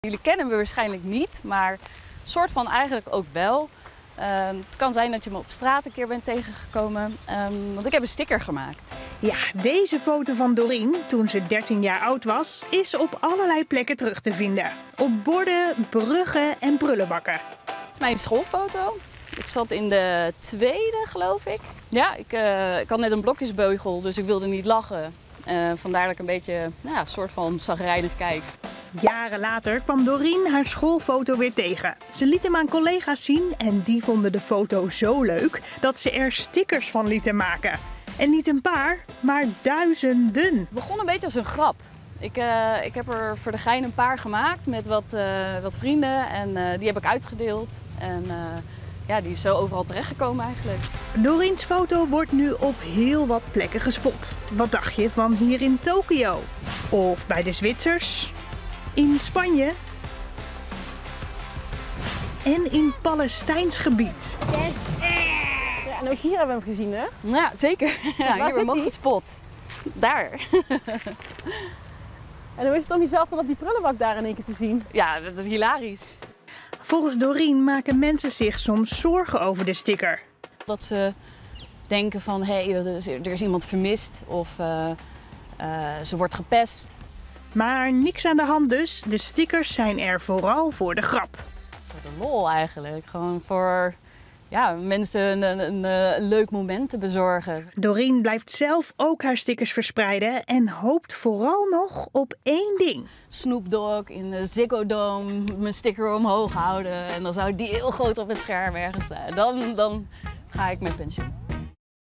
Schoolfoto van Dorien (13) hangt op verkeersborden over de hele wereld
Hoe vind jij het om op de schoolfoto te gaan? Dorien uit Den Haag vond het verschrikkelijk toen ze 13 jaar was. Ze had toen net een blokjesbeugel en wilde niet lachen. Die schoolfoto van haar hangt over de hele wereld!